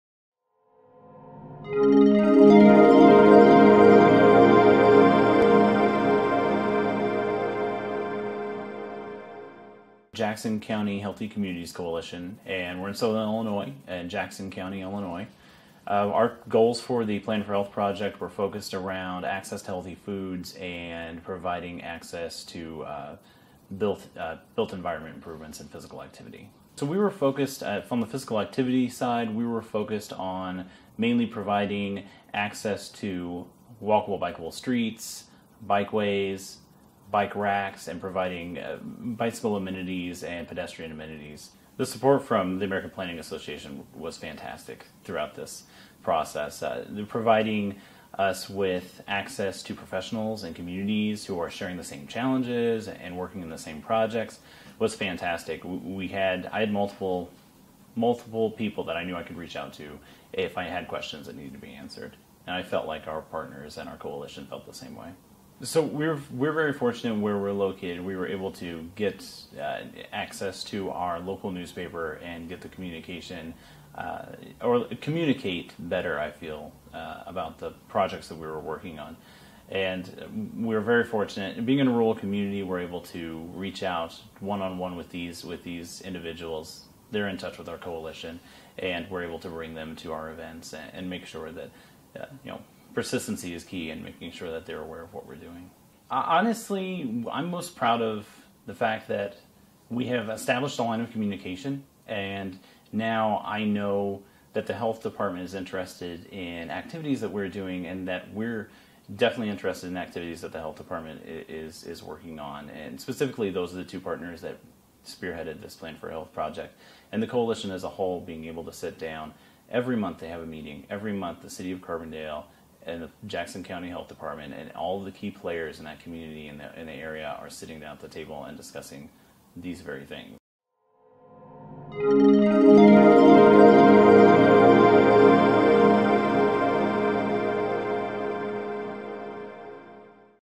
Establishing a Line of Communication: Travis Taylor on Plan4Health
Travis Taylor, AICP, discusses the Plan4Health project in Jackson County, Illinois.

For more information on how APA works to plan for public health, visit the Planning and Community Health Center